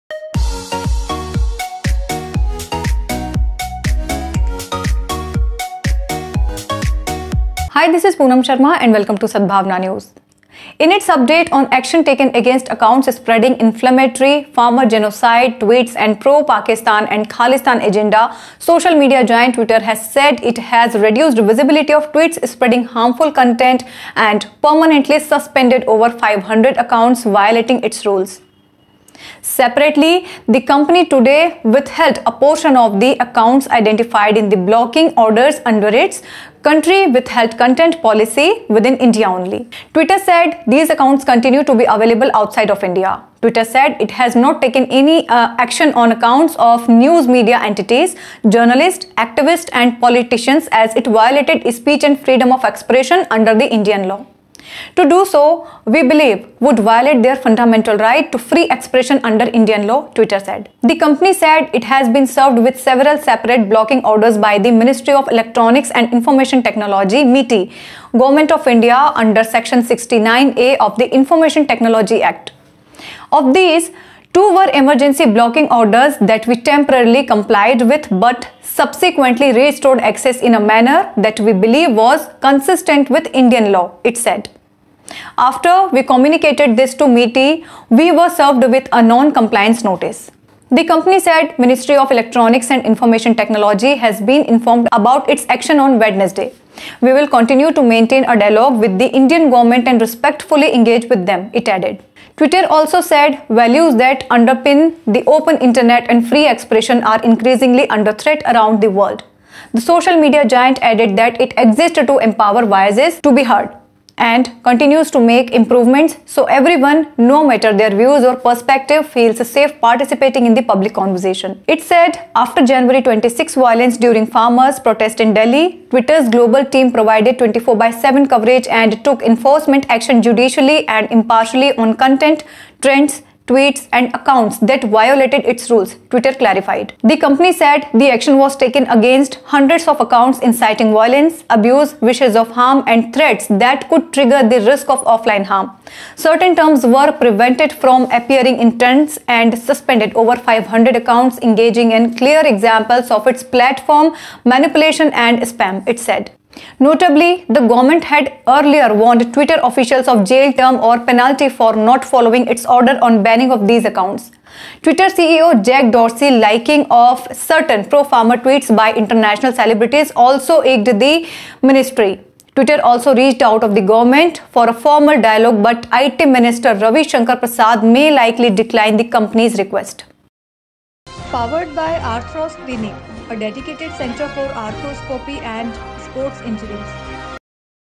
Twitter starts blocking censored accounts after govt's diktat
Over 500 accounts suspended, no action on accounts of journalists, activists, politicians: Twitter

Twitter says "values that underpin the Open Internet and free expression" are increasingly under threat around the world; lists action taken against flagged accounts after govt's non-compliance notice

In its update on action taken against accounts spreading inflammatory "farmer genocide" tweets and pro-Pakistan and Khalistan agenda, social media giant Twitter has said it has reduced visibility of tweets spreading "harmful content" and permanently suspended over 500 accounts violating its rules.

Separately, the company today withheld "a portion" of the accounts identified in the blocking orders under its 'Country Withheld Content' policy within India only. Twitter said these accounts continue to be available outside of India. Twitter said it has not taken any action on accounts of news media entities, journalists, activists, and politicians as it violated "speech and freedom of expression" under the Indian law. "To do so, we believe, would violate their fundamental right to free expression under Indian law," Twitter said.

The company said it has been served with several separate blocking orders by the Ministry of Electronics and Information Technology (MeitY), Government of India, under Section 69A of the Information Technology Act. Of these, two were emergency blocking orders that we temporarily complied with but subsequently restored access in a manner that we believe was consistent with Indian law, it said. "After we communicated this to MeitY, we were served with a non-compliance notice."

The company said MeitY (Ministry of Electronics and Information Technology) has been informed about its action on Wednesday. "We will continue to maintain a dialogue with the Indian government and respectfully engage with them," it added.

Twitter also said "values that underpin the Open Internet and free expression" are increasingly under threat around the world. The social media giant added that it "exists to empower voices to be heard", and continues to make improvements so everyone - no matter their views or perspective - feels safe participating in the public conversation.

It said after January 26 violence during farmers' protest in Delhi, Twitter's global team provided 24/7 coverage and took enforcement action judiciously and impartially on content, trends, tweets, and accounts that violated its rules, Twitter clarified.

The company said the action was taken against hundreds of accounts inciting violence, abuse, wishes of harm, and threats that could trigger the risk of offline harm. Certain terms were prevented from appearing in trends and suspended over 500 accounts engaging in clear examples of its "platform manipulation and spam", it said.

Notably, the government had earlier warned Twitter officials of jail term or penalty for not following its order on banning of these accounts. Twitter CEO Jack Dorsey's liking of certain pro-farmer tweets by international celebrities also irked the ministry. Twitter also reached out to the government for a formal dialogue but IT minister Ravi Shankar Prasad may likely decline the company's request.